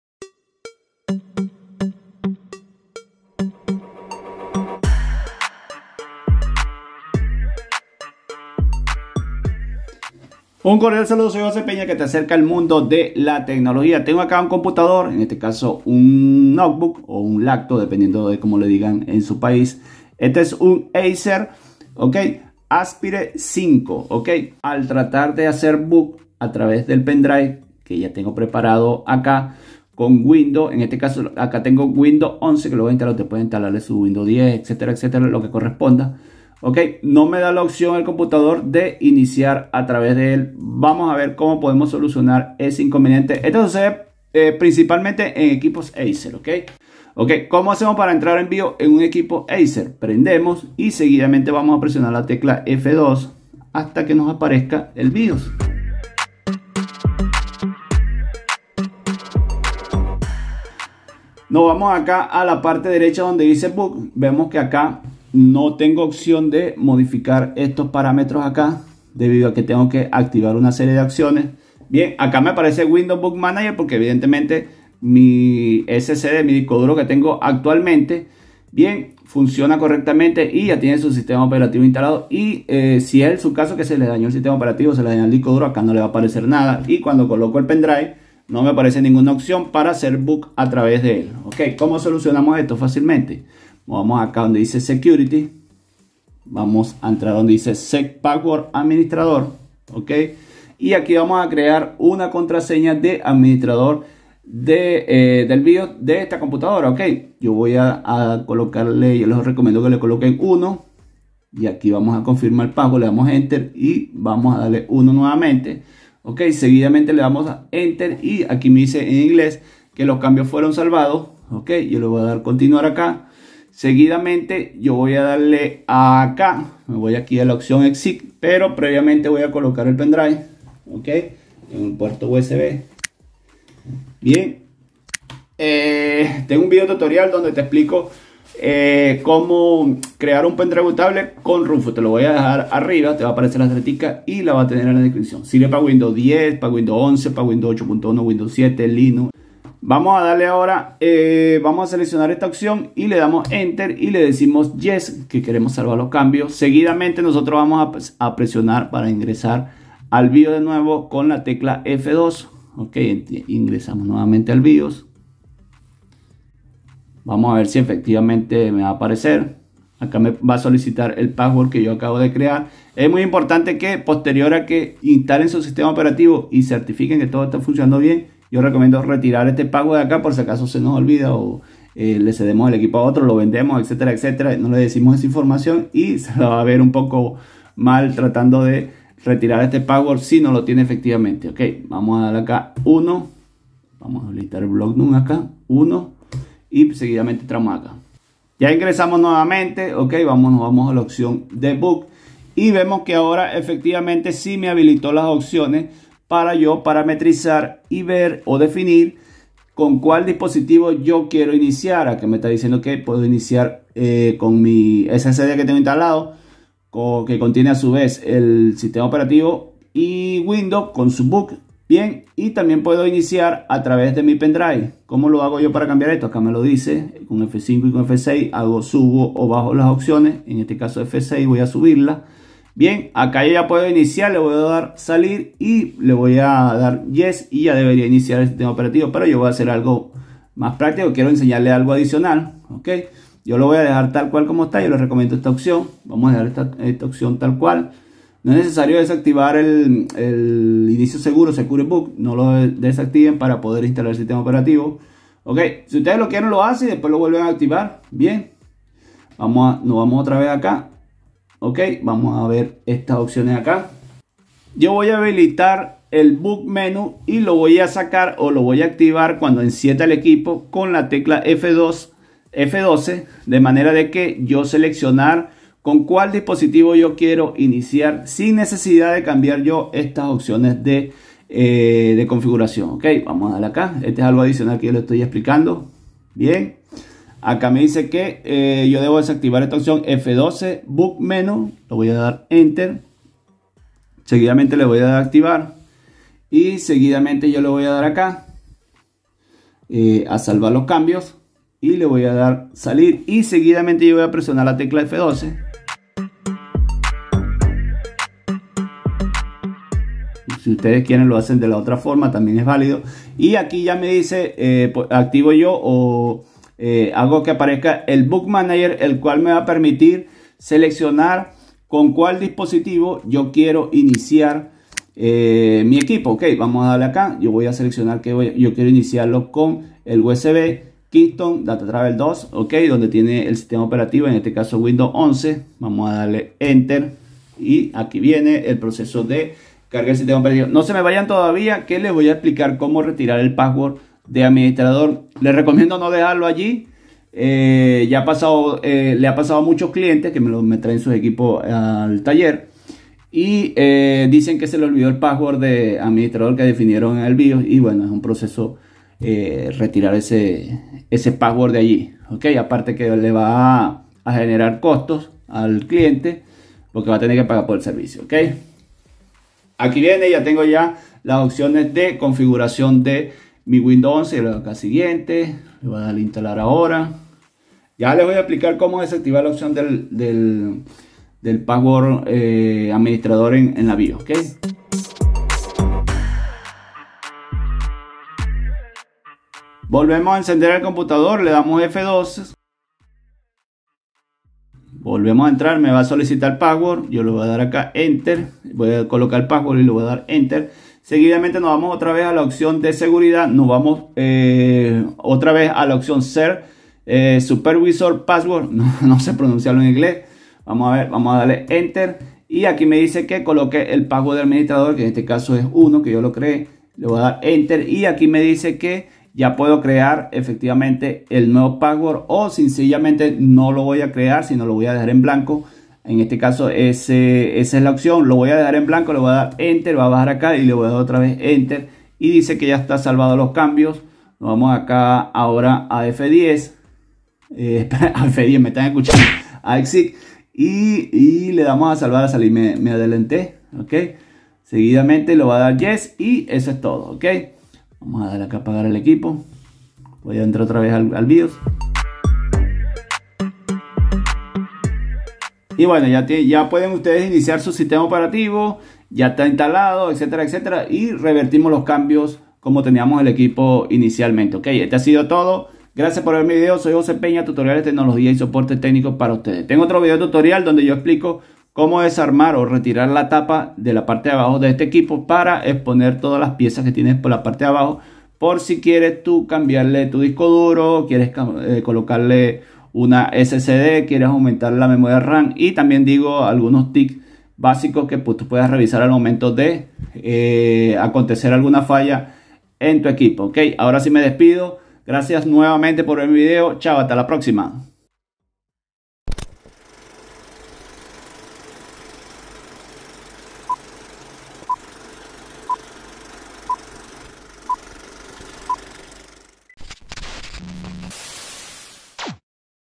HABILITAR EN BIOS OPCIONES BLOQUEADAS DE ARRANQUE con Pendrive de WINDOWS en Acer Aspire💻
👉SOLUCIÓN👈 para cuando esta deshabilitado para iniciar con Pendrive Booteable de Windows 10 o 11 en Laptop💻 ACER ASPIRE 5, ademas también se soluciona cuando no aparece el Menu Boot Manager en estos equipos.
Video aplica para los modelos con BIOS similares, en este caso es la versión InsydeH20 Rev. 5.0
Puedes basarte en este tutorial para las ACER ASPIRE 5 en general ya que son muy similares a este modelo.
Modelo mostrado en el tutorial: Aspire 5 A515-54
Boot Fail and Install from usb Windows 10 and 11 on Acer Aspire 5
Videos Cortos 👆Tecnológicos que te harán la vida más fácil.
👉👉👉👉Página Web Oficial del Canal👈👈👈👈